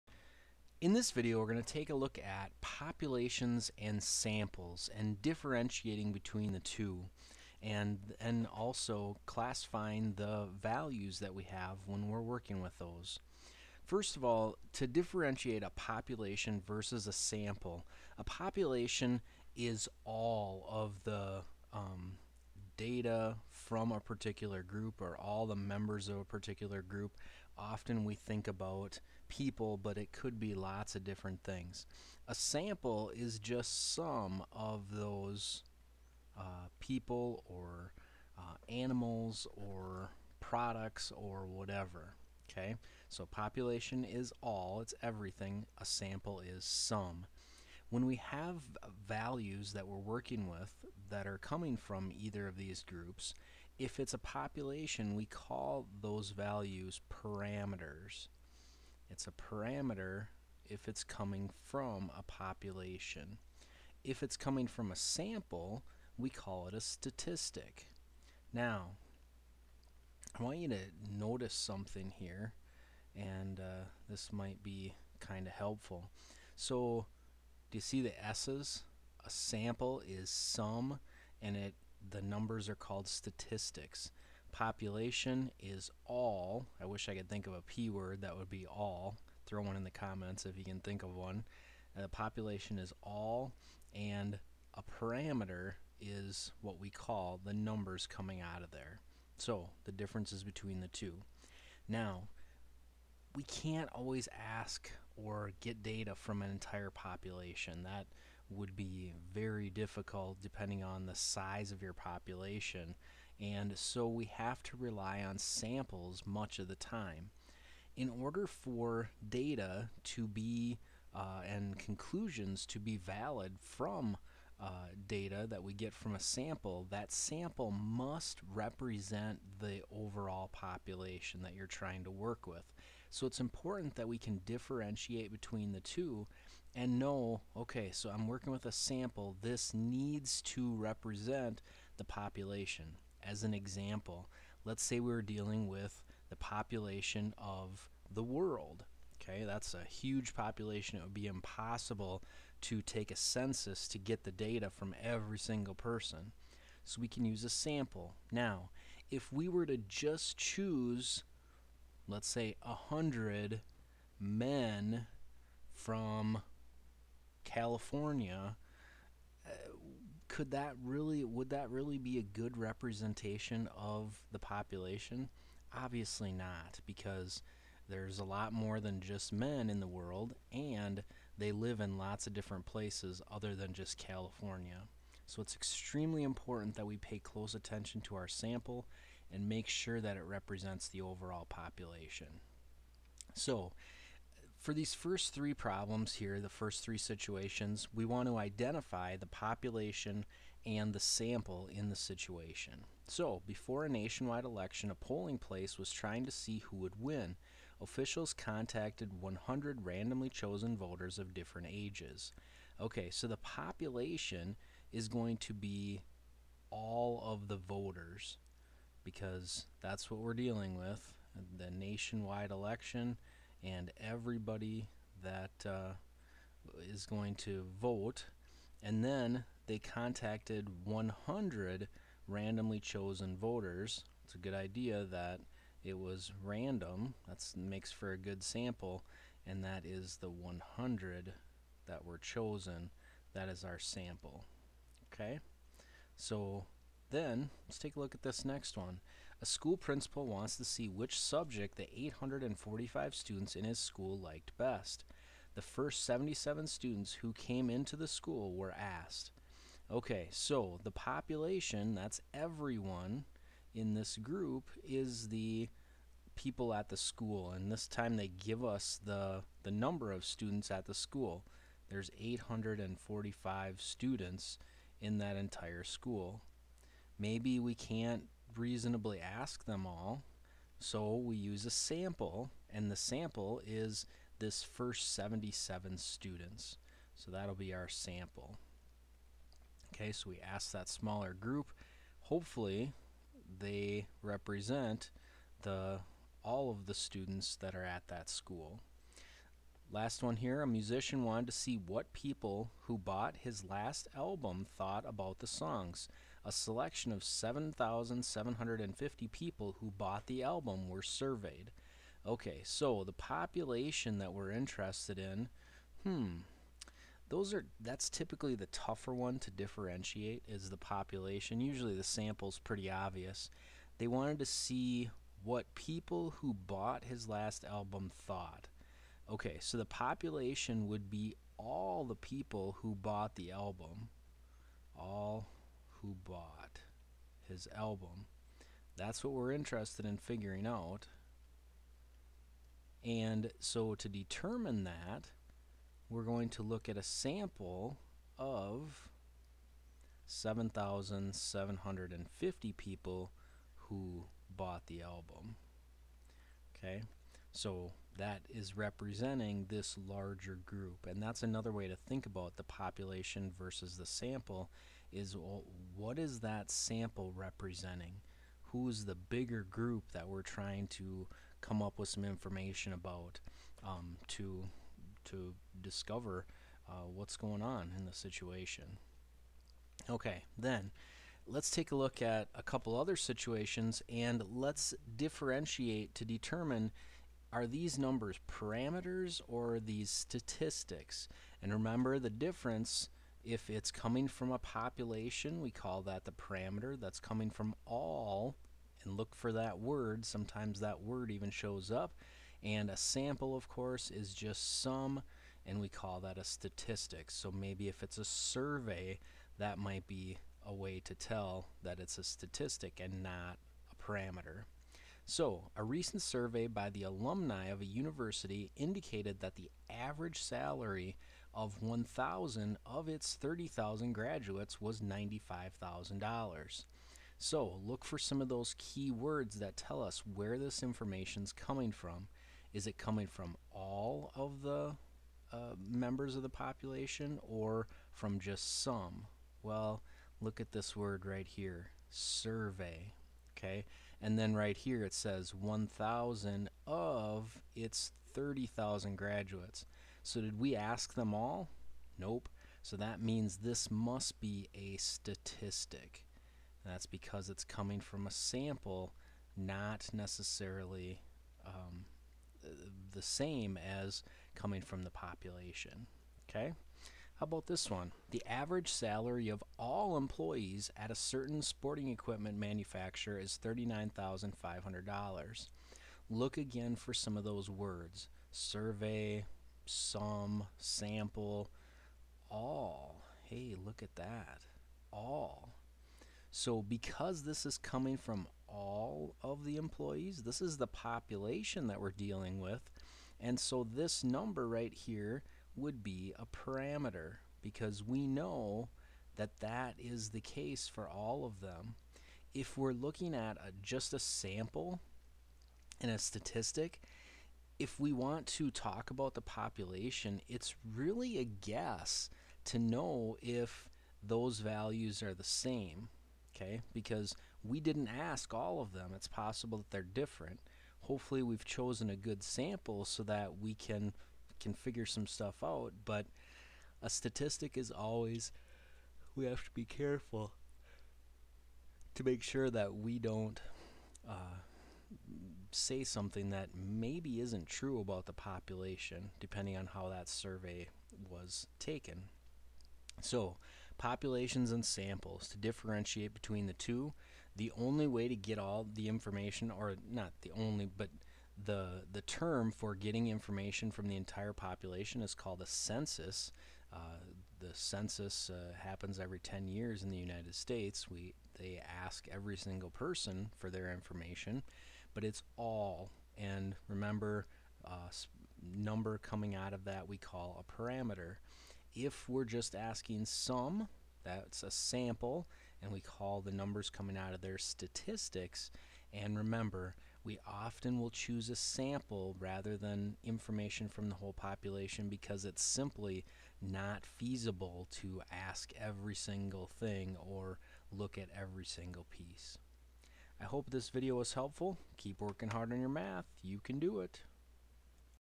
Populations and Samples
This video looks at identifying populations and samples from situations.  Classifying parameters and statistics depending on where a number comes from is also covered.  This video includes 5 example problems.